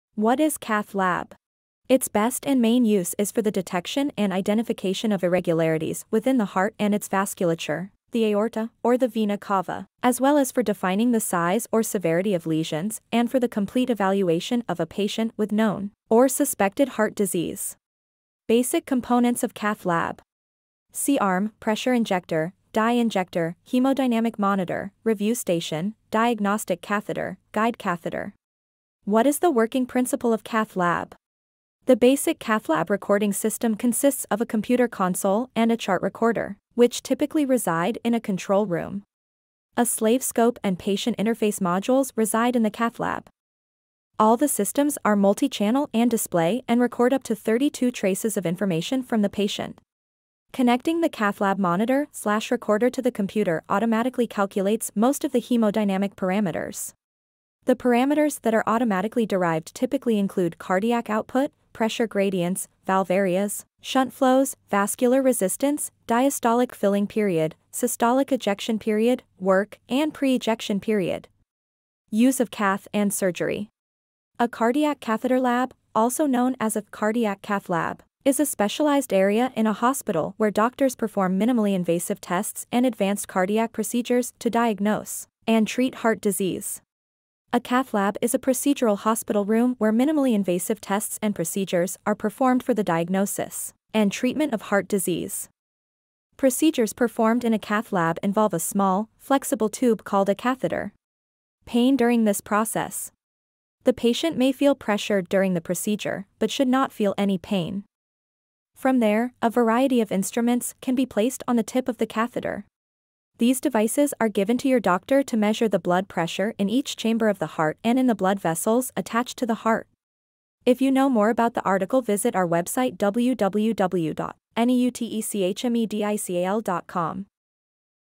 What is Cath Lab | Cath Lab Procedures | Cath Lab basics | Cath Lab Machine View
In this video, we'll take you on a journey through the cath lab, explaining the basics of this critical medical facility. Whether you're a medical professional, a student, or simply curious about cardiovascular health, this video is for you.

What You'll Learn:

1. What is a Cath Lab?: Understand the purpose and function of a cath lab, including its role in diagnosing and treating cardiovascular diseases.
2. Cath Lab Procedures: Learn about the various procedures performed in a cath lab, including angiograms, angioplasty, stenting, and more.
3. Cath Lab Basics: Get familiar with the basic equipment and tools used in a cath lab, including the cath lab machine.
4. Cath Lab Machine View: Take a closer look at the cath lab machine and understand its components and functions.
5. Real-Life Examples: See real-life examples of cath lab procedures and understand how they're performed.

Its best and main use is for the detection and identification of irregularities within the heart and its vasculature, the aorta, or the vena cava, as well as for defining the size or severity of lesions and for the complete evaluation of a patient with known or suspected heart disease. It is also used in the diagnosis of disorders such as reduced left ventricular function, valve dysfunction, pulmonary vascular disease and congenital anomalies and is also used for pacemaker lead placement.